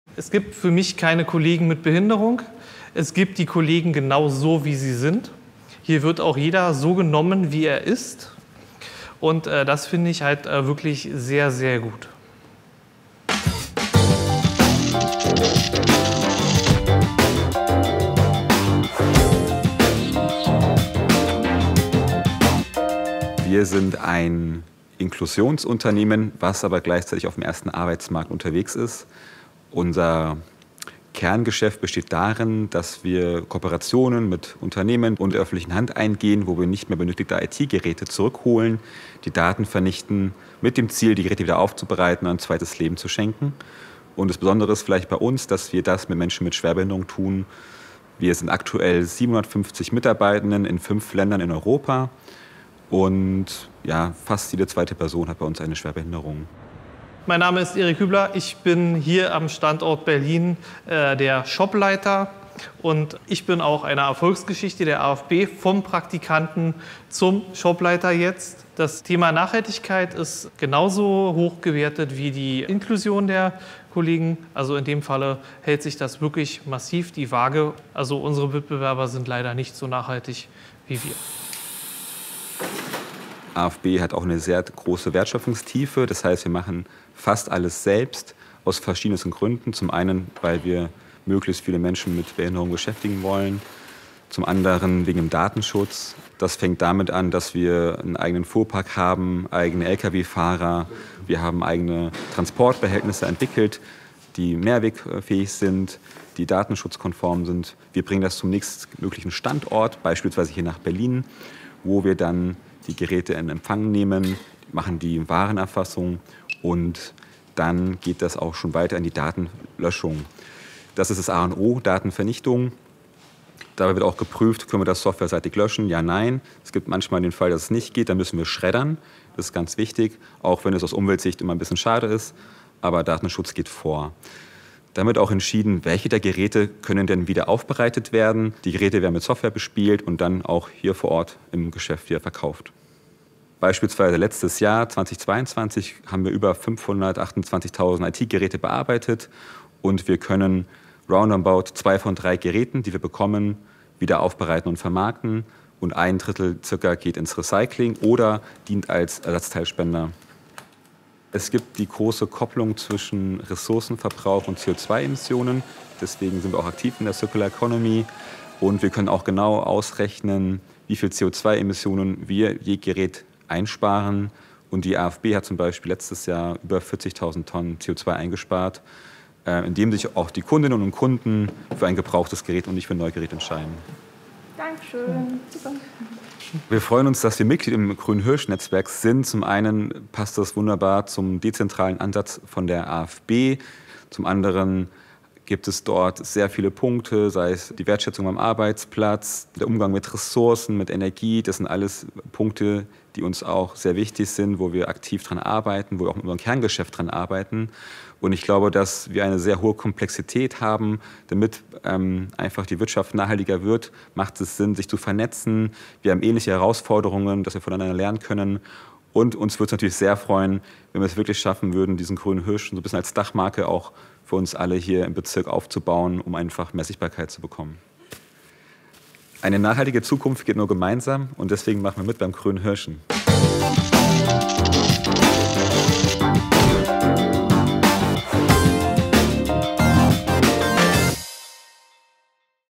AfB social & green IT - nachhaltiges IT-Unternehmen
Das IT Unternehmen AfB gGmbH beschäftigt sich mit dem sogenannten Refurbishing von Firmen-Hardware. Das bedeutet, dass die Mitarbeitenden gebrauchte IT- und Mobilgeräte aufarbeiten und anschließend wiedervermarkten. Das einzigartige dabei: All das wird hier von Menschen mit und ohne Behinderung gemacht. So sollen in Europa 500 Arbeitsplätze für behinderte Menschen geschaffen werden - mehr als die Hälfte davon ist bereits erreicht. Ein tolles Beispiel, wie Inklusion auf dem ersten Arbeitsmarkt und Nachhaltigkeit zusammengedacht werden können.